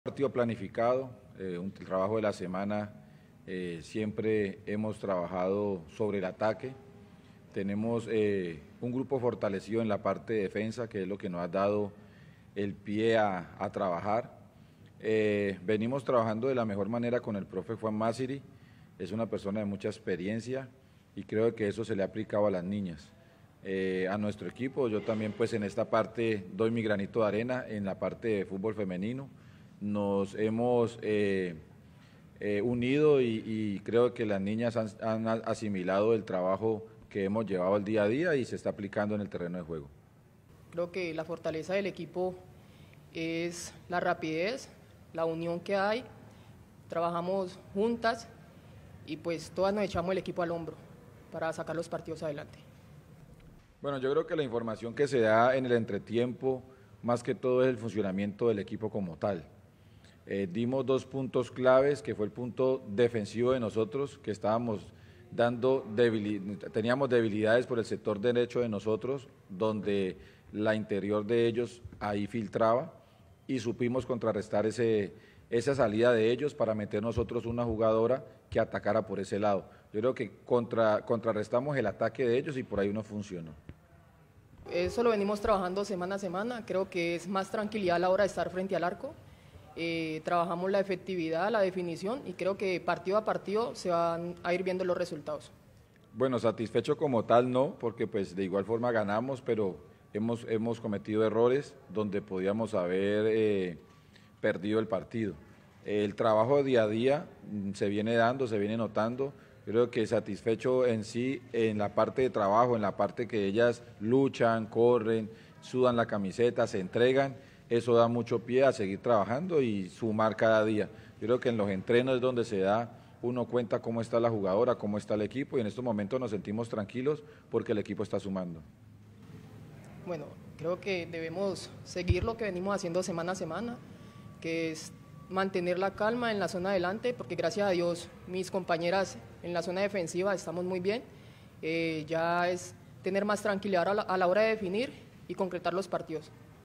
Alianza FC femenino lleva 4 fechas sin conocer la derrota, venció a Llaneros FC.
Con gol de Sindy Sánchez en el minuto 65, Alianza FC venció de visitante a Llaneros cero a uno, resultado que puso a soñar a las yumbeñas quienes llevan cuatro fechas sin conocer la derrota.